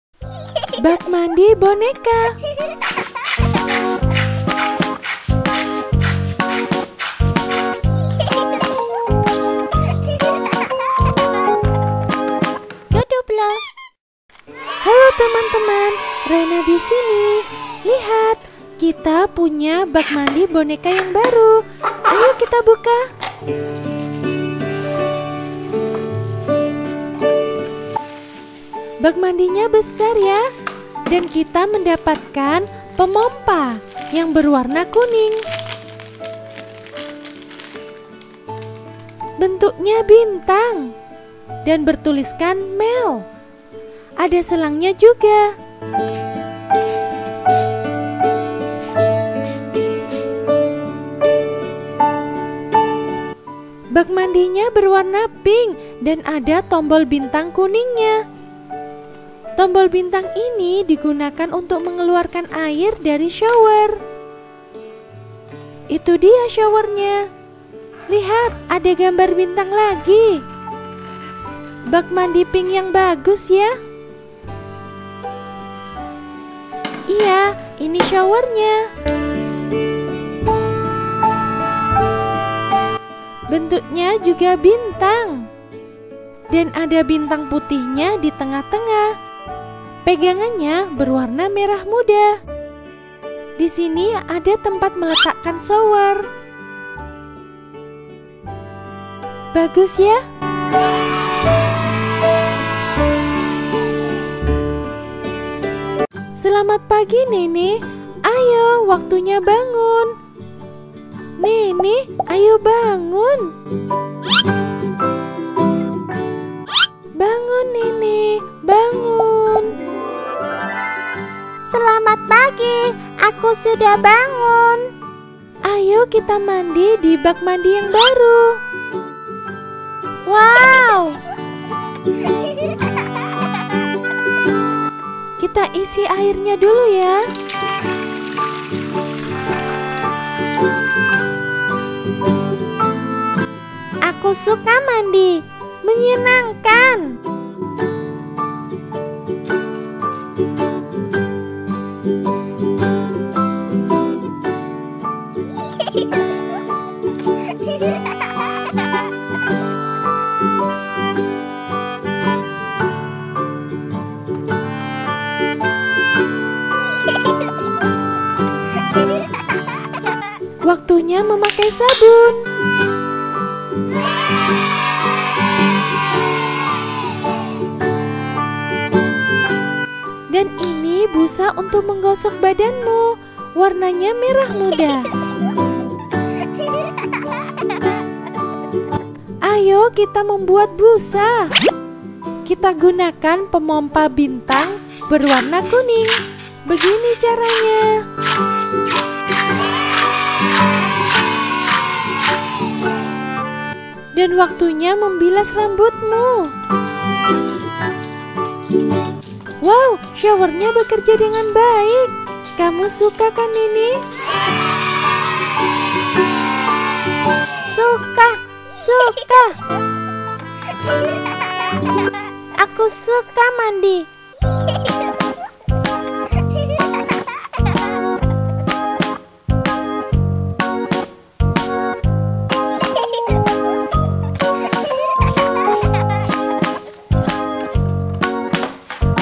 Mainan Boneka Eps 6 Bak Mandi Boneka Unboxing - GoDuplo TV
Mainan Boneka Eps 6 Bak Mandi Boneka Unboxing - Bathub Set - GoDuplo TV. Kartun Diva Kastari Animation.
Ayo cepat bangun Nene, karena ini sudah pagi. Waktunya bangun dan mandi di bak mandi boneka yang baru. Bagus sekali, ada shower nya, dan busa nya bisa banyak sekali.
Season 1-P1 Perkenalan